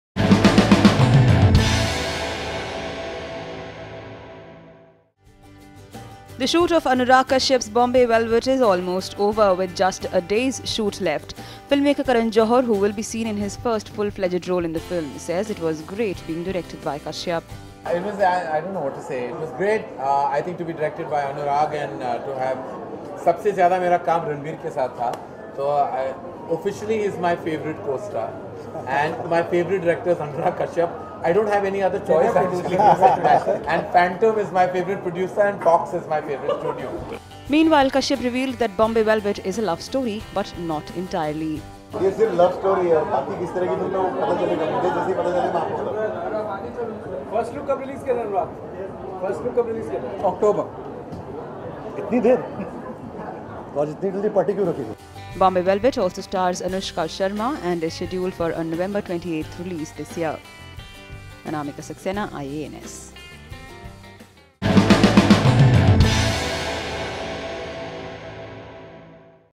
Karan Johar in a negative role in Bombay Velvet - IANS India Videos
Filmmaker Karan Johar, who will be seen in his first full-fledged role in Bombay Velvet, says it was great being directed by Anurag Kashyap.